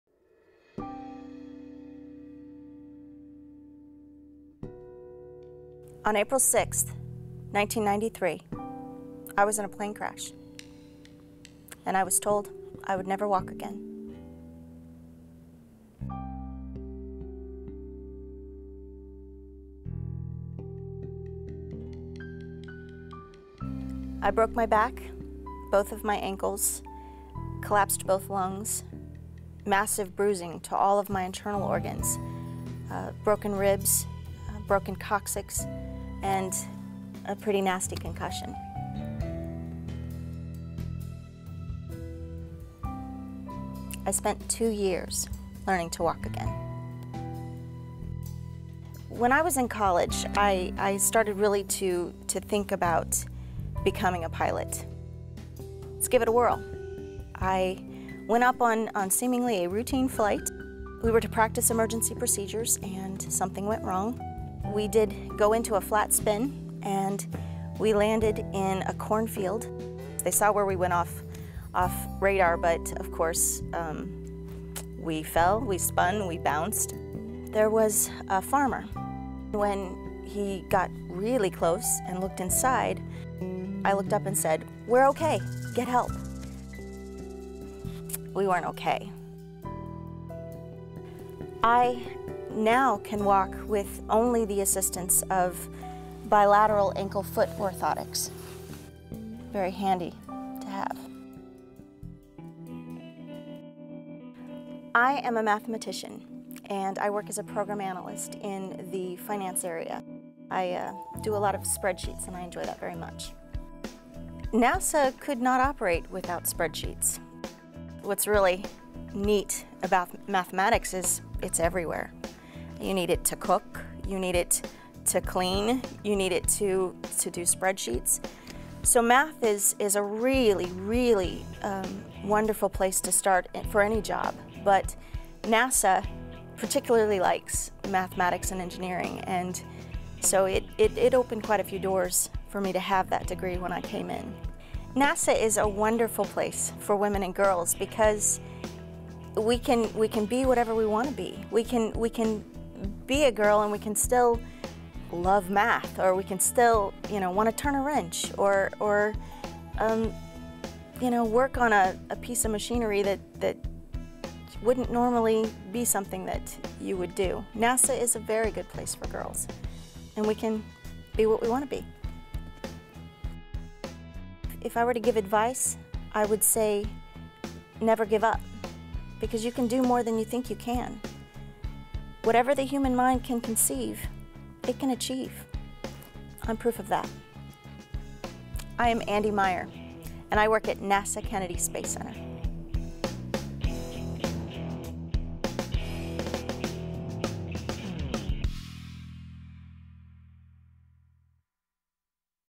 Andrea Meyer
Andrea Meyer, a Program Analyst for NASA talks about her experiences working at Kennedy Space Center in this Women@NASA profile.